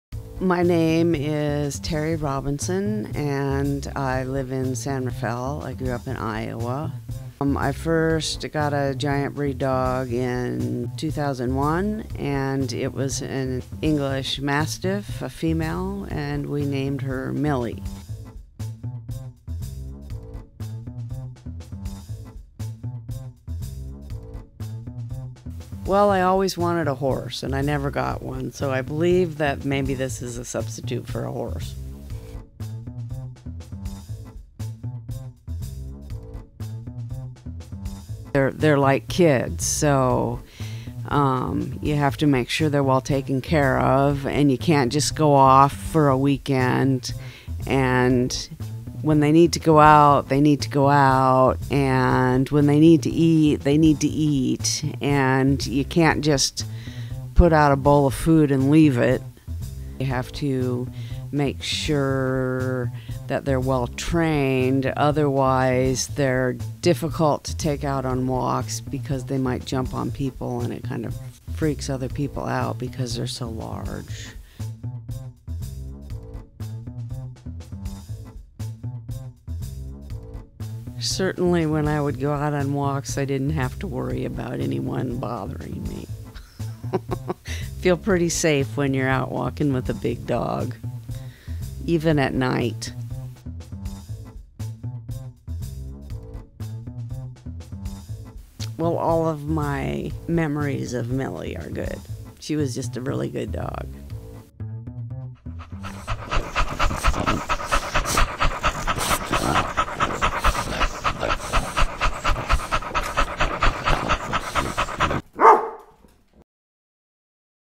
Big Dog Living Trailer
Final project trailer for my LMW 1 class, for my website about living with giant breed dogs.